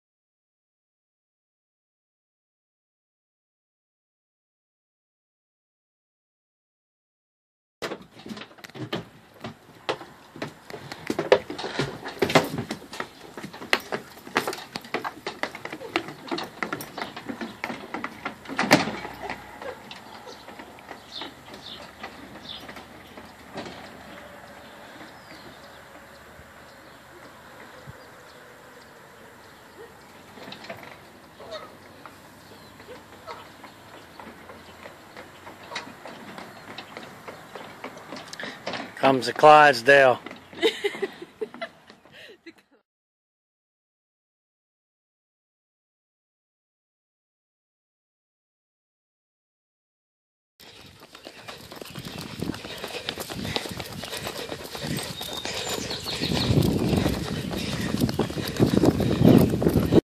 Scootie's New Booties!
See Scooter go from walking like a Clydesdale to running like a champ!  She loves her new booties!